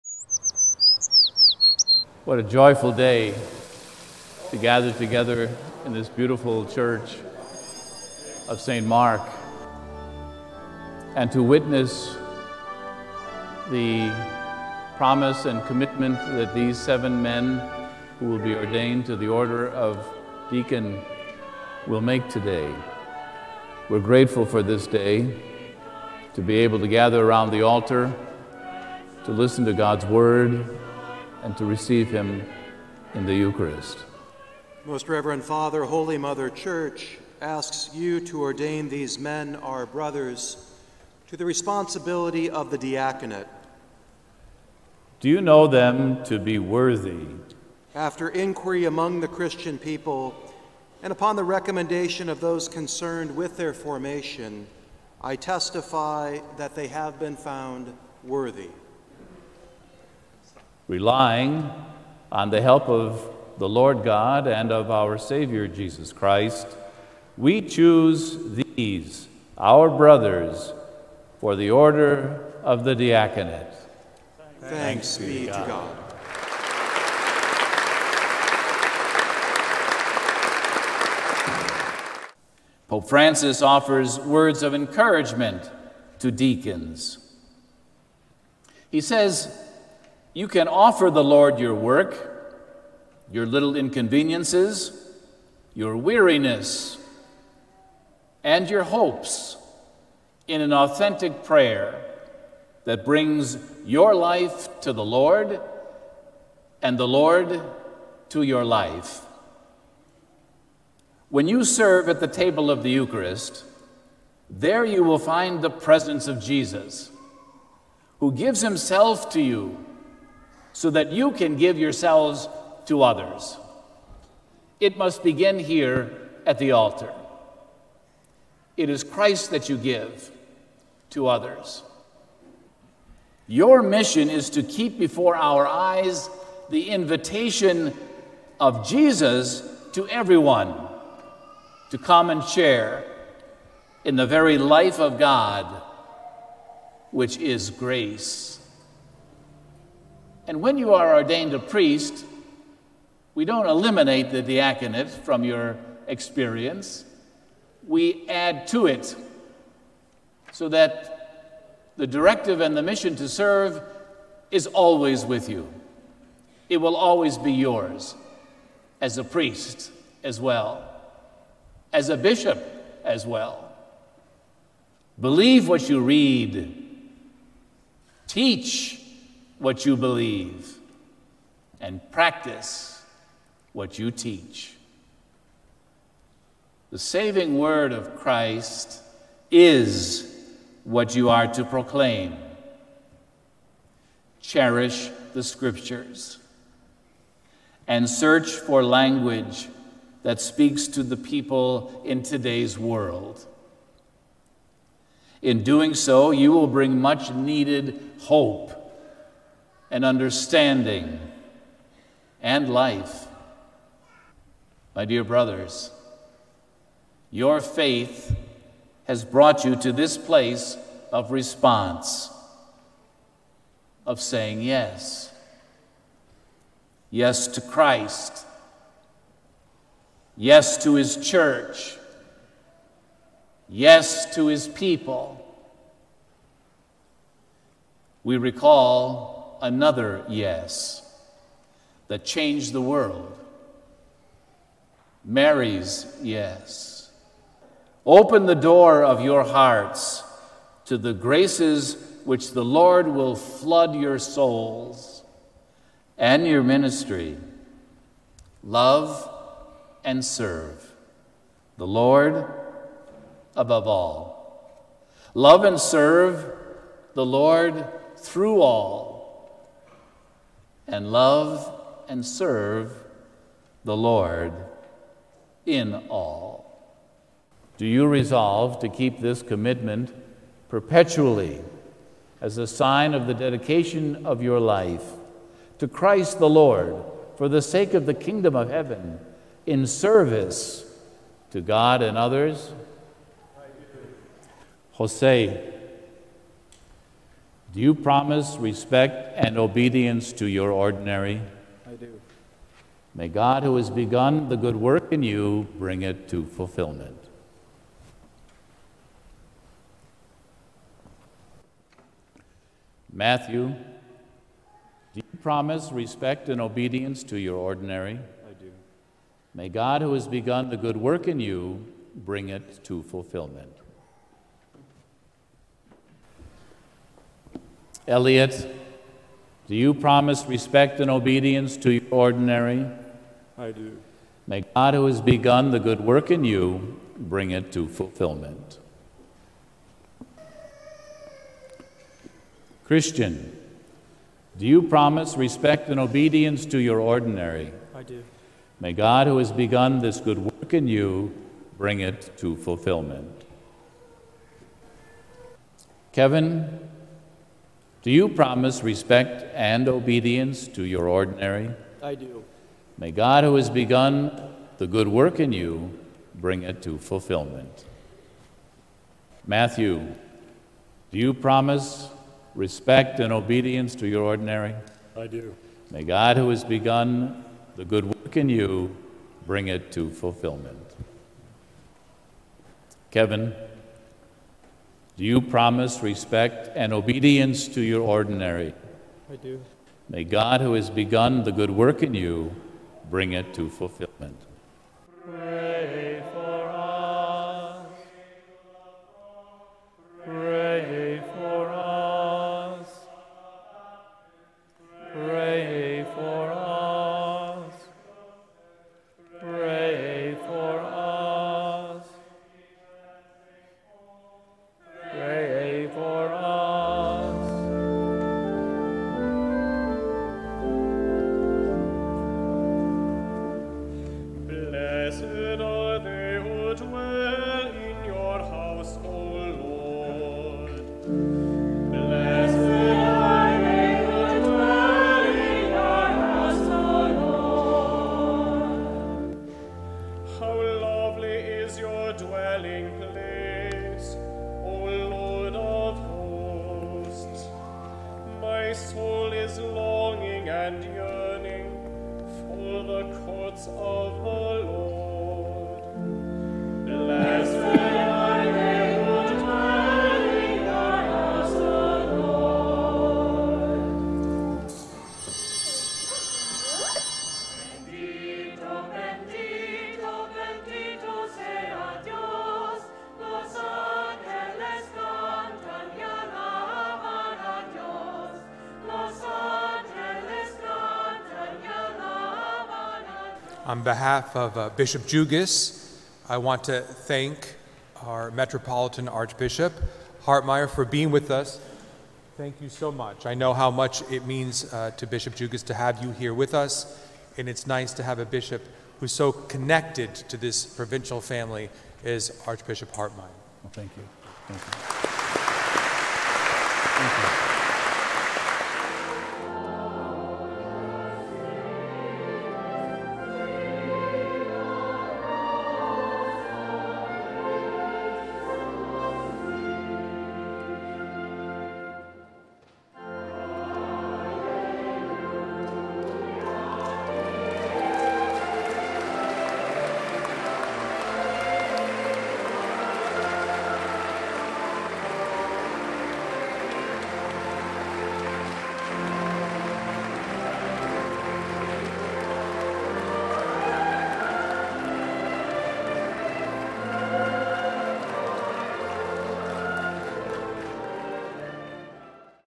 Seven men ordained deacons: highlight video
One step closer to the priesthood, seven men were ordained deacons on June 3, 2023 at St. Mark Church in Huntersville. The men will continue their studies in hopes of being ordained priests in 2024.
The newly ordained deacons are Matthew Wayne Dimock Jr., Christian Joseph Goduti, Matthew Philip Harrison II, Kevin Ruben Martinez, José Alfredo Palma Torres, Elliott Cade Suttle and Kevin Michael Tran.